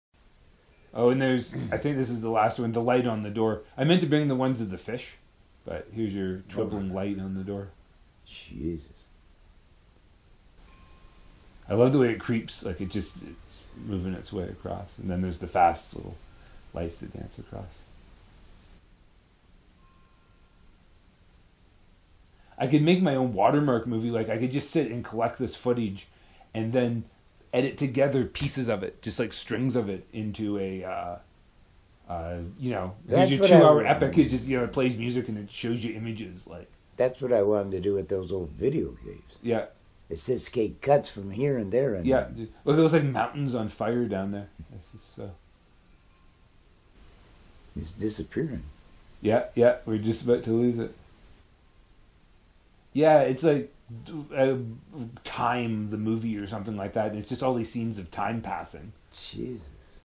Postcards and $hit with Jack and Dylon - Episode #188 - Twibbling Light
Not sure where I first heard the word “twibbling” from. But I swear that’s how Jack Whyte​ once described the dancing sunlight as it filtered through the trees in the yard outside his studio/home onto the walls inside. Of course, he denies existence of the word "twibbling" as we regularly watch spectacular mini light shows indicative of this video unfold.